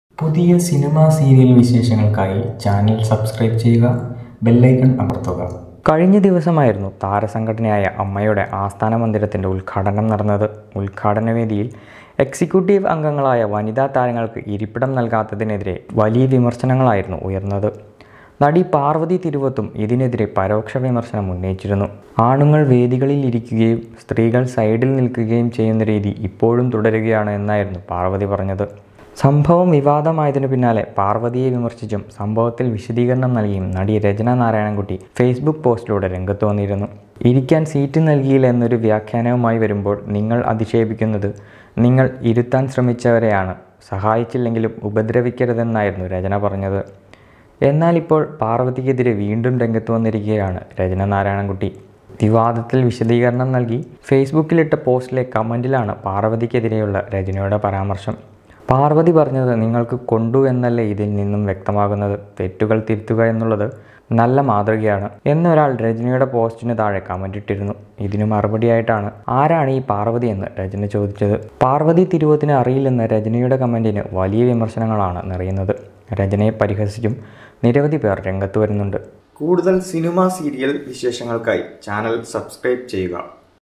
സോഷ്യല്‍ മീഡിയയില്‍ പാര്‍വ്വതി തിരുവോത്തുമായി ഏറ്റ് മുട്ടി രചന നാരായണന്‍കുട്ടി| rachana​ narayanan
സോഷ്യല്‍ മീഡിയയില്‍ പാര്‍വ്വതി തിരുവോത്തുമായി ഏറ്റ് മുട്ടി രചന നാരായണന്‍കുട്ടി